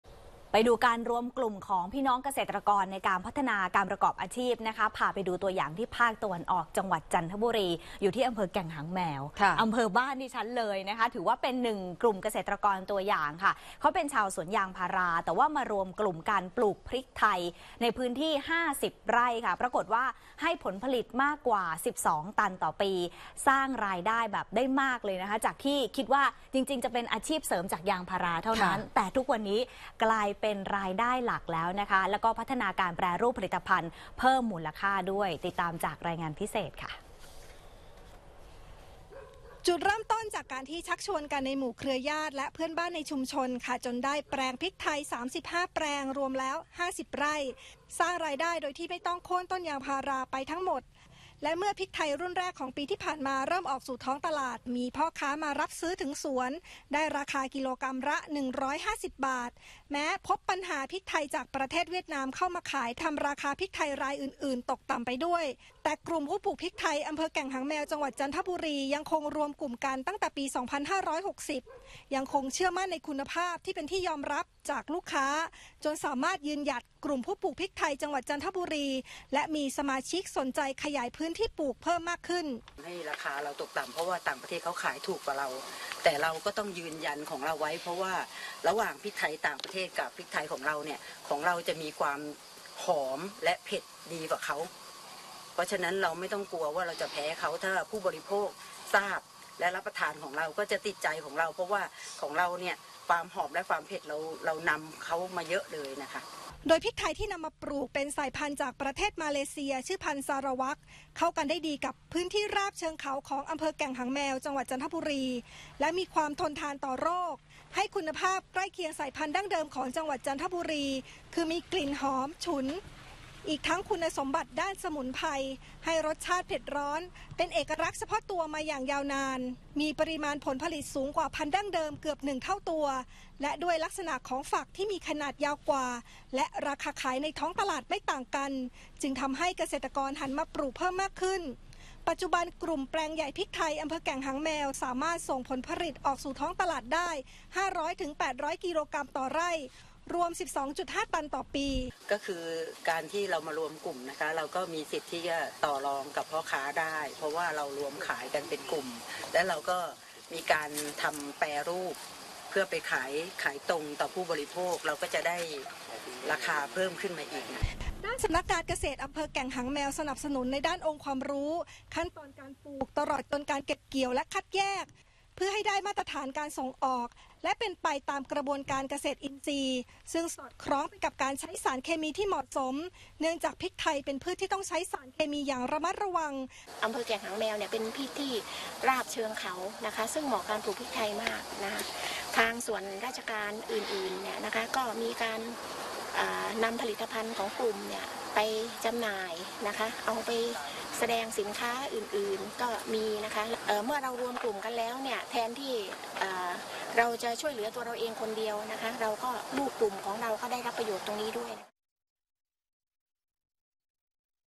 สวนยางจันทบุรี รวมกลุ่มปลูกพริกไทยให้ผลผลิตมากกว่า12ตันต่อปี พลิกสร้างรายได้งาม
รายงานพิเศษ NBT 21 ก.ย. 2561